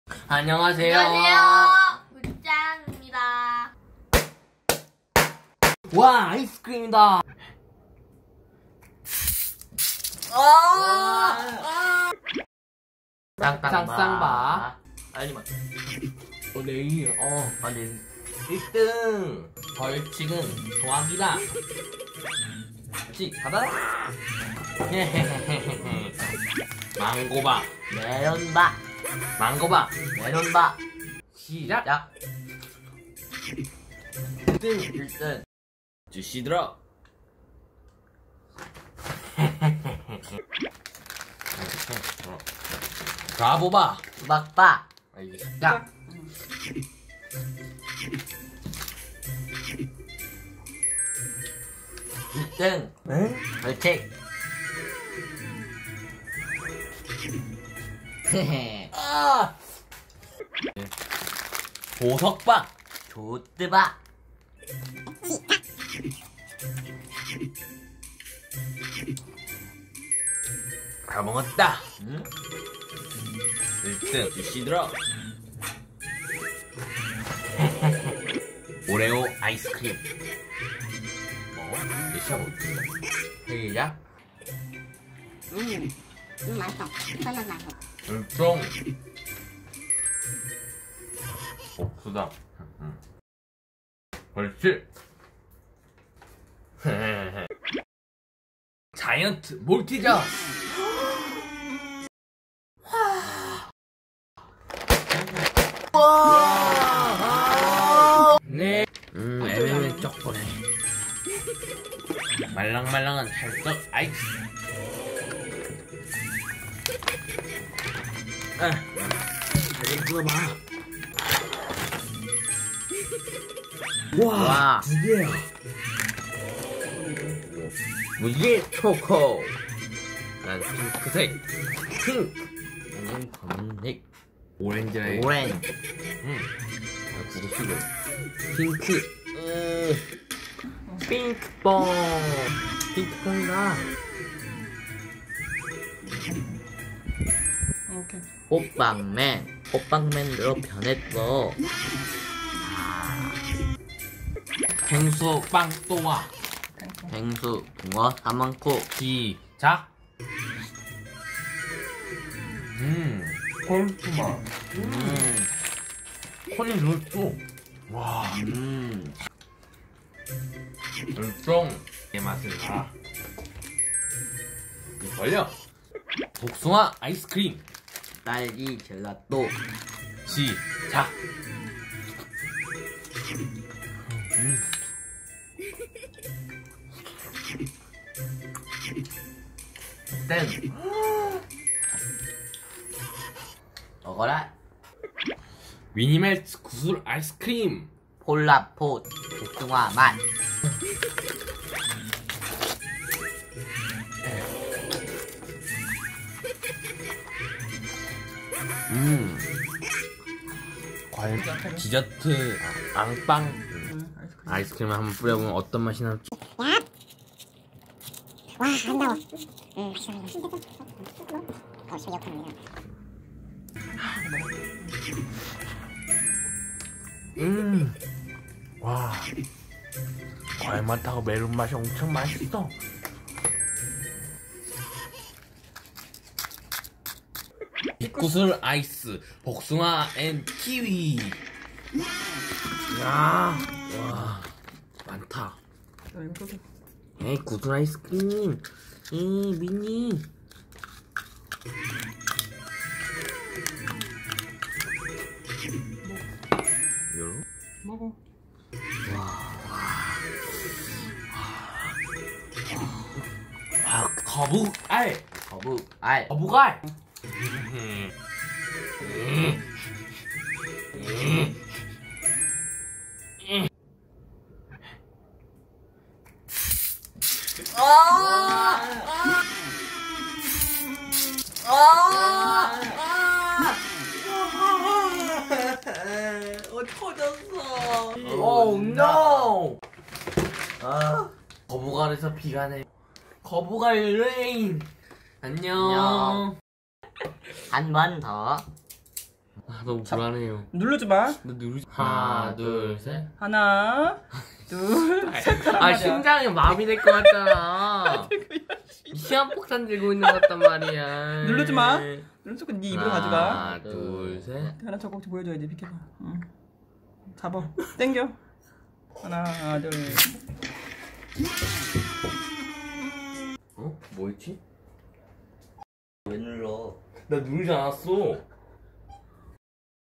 ASMR ICE CREAM PARTY아기상어 거북알등 다양한 아이스크림 격투먹방 GREEN TEA CHOCOLATE MAGNUM EATING SOUNDS 咀嚼音 モッパン
[굿짱TV]다양한 아이스크림을 가지고 먹방 대결을 해 보았어요.
좋아요 구독은 저에게 큰 힘이 됩니다.
We had an eating show contest with a variety of ice cream.

Blowing up
twin bar
QUICK BLAST(red blue) Sour Spray
mango bar
Meron ice cream
Juicy Drop
Burabo Ice Cream Green Tea Taste
Watermelon
Mega mouse
jewelry bar
shark ice cream
Oreo Ice Cream
Giant Maltese
Balloon-made Maltese
M&M chocolate
Maltisers
Pink Fong Baby Shark Making Perfect Ice Cream
Hoppang Man Making Chalddeok Ice Cream
a soft, smooth-fitting piece of ice.
Eating Rainbow Chocolate Art Pan
Pengsu Breadthoa Ice Cream
Pengsu bungeo-ssamanko green tea flavor
peach ice cream
strawberry gelato ice cream
Minimelz bead ice cream
Polaroid strawberry flavor
dessert bread
melon ice sand
Big marble peach kiwi ice cream